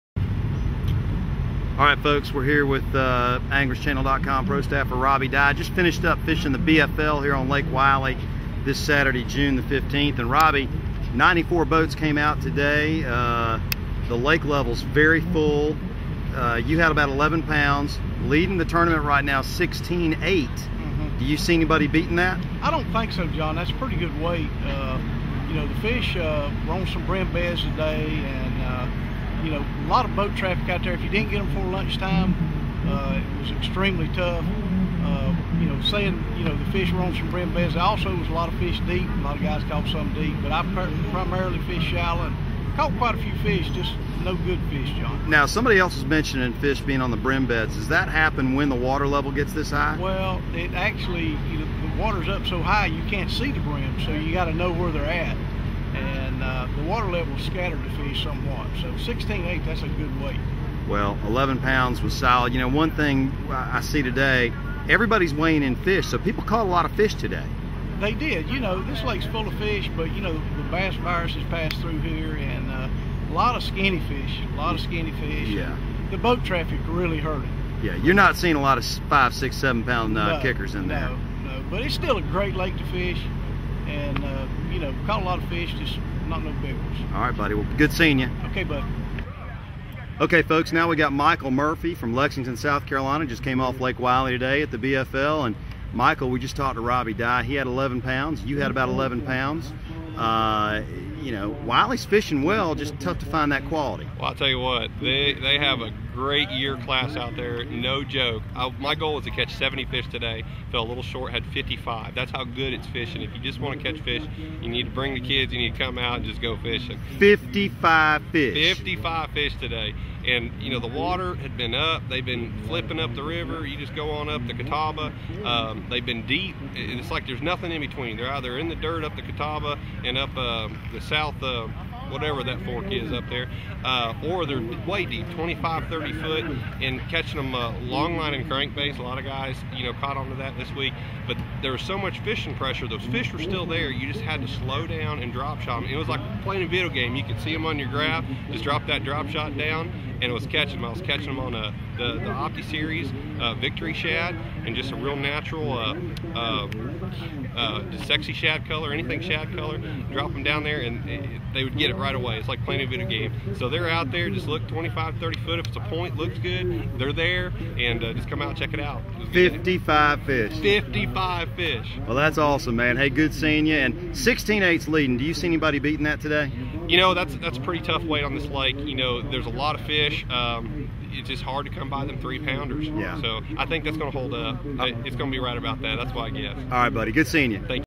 South Carolina BFL Lake Wylie Weigh-in Highlights with Rob D
AC Pro Staffer Rob Digh as well as FLW Tour Pro Michael Murphy drop by after weighing in 11 Lbs each at Saturdays BFL on Lake Wylie. As you will hear each angler say, catching fish is not the problem on Wylie at all, just getting that quality bite to get over the top is the key!